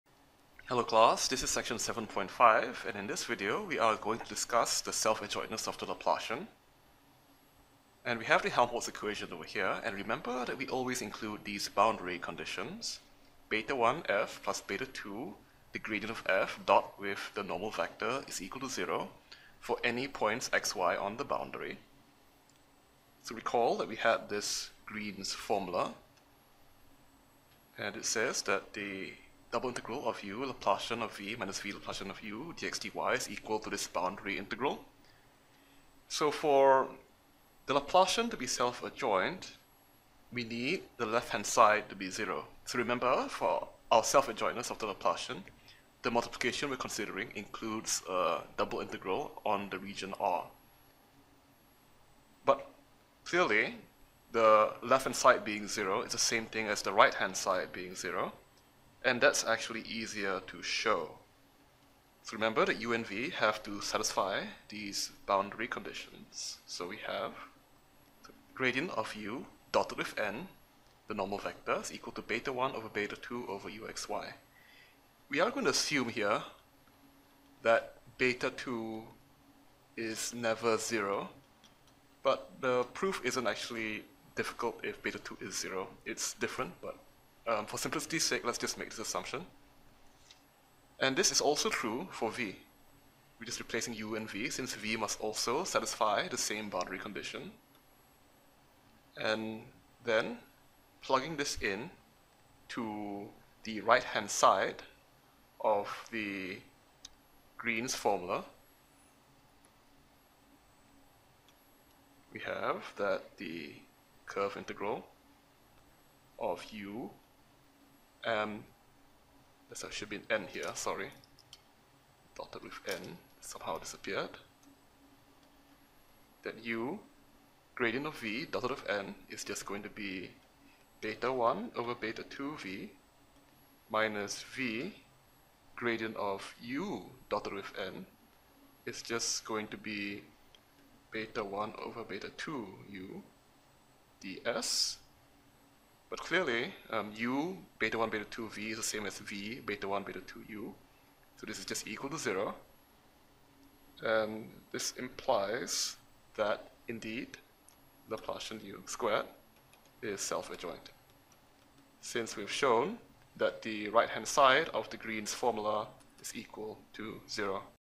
Self-adjointness of the Laplacian
We prove self-adjointness of the Laplacian given appropriate boundary conditions